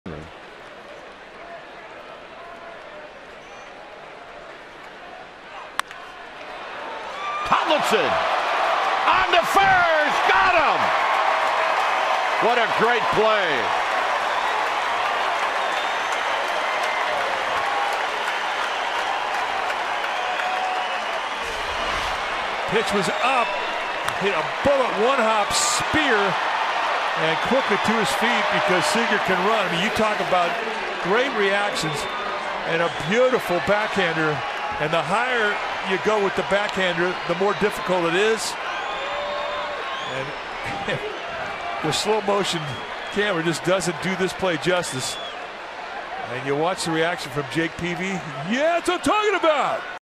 LAD@SF: Tomlinson dives, throws out Seager at first
9/28/15: Kelby Tomlinson lays out to his right, then gets up and throws to first to get Corey Seager in the 6th